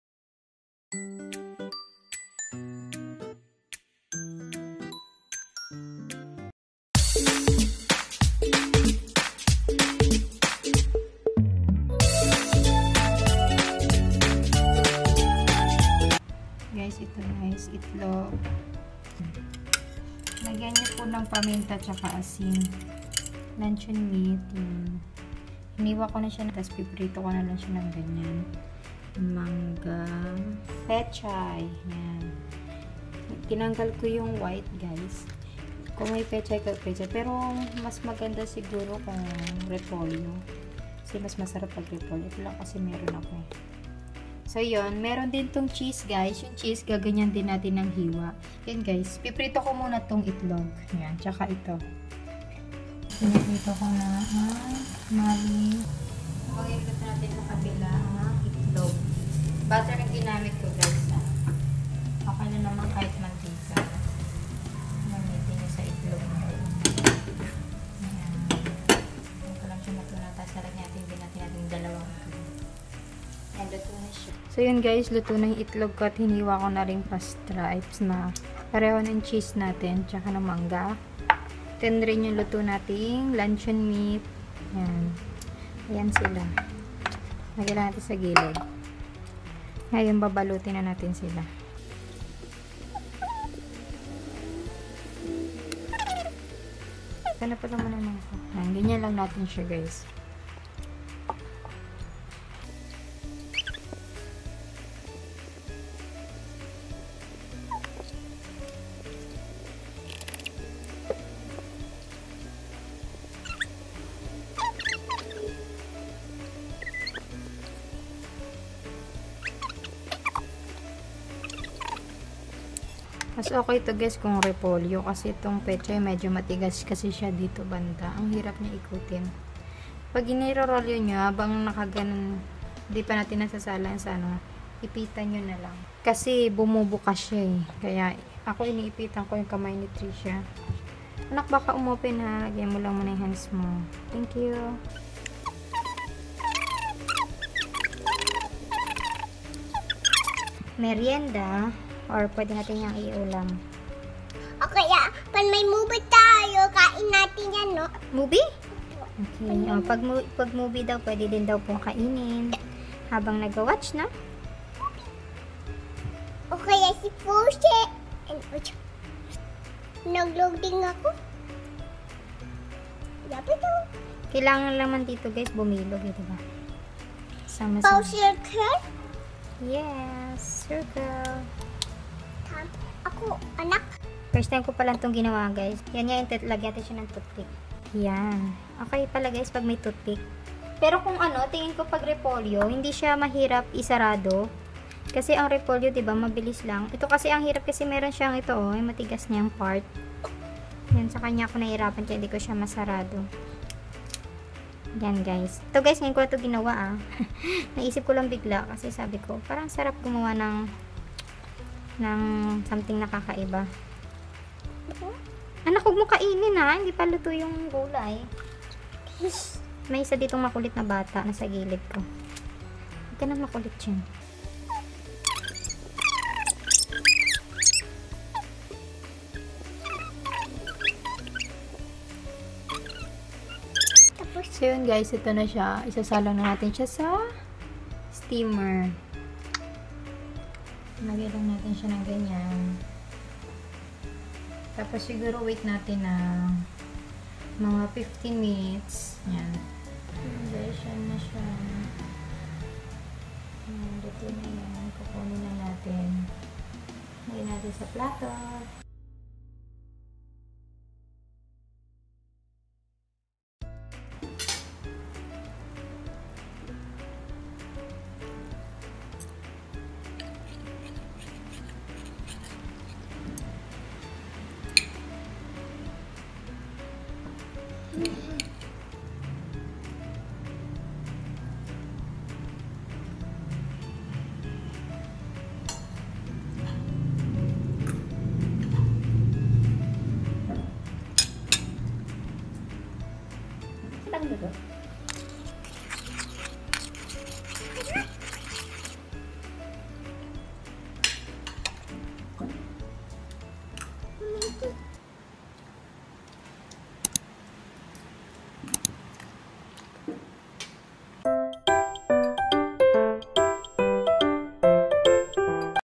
Pechay Roll | Luncheon Meat with Mango | quarantinefood (ayudang delata) | mhaiiCooks | TMTCvids
Ingredients :
Pechay
Luncheon Meat
Egg
Cheese
Mango
Butter/oil

Dipping sauce:(optional)
Mayonnaise
Salt&pepper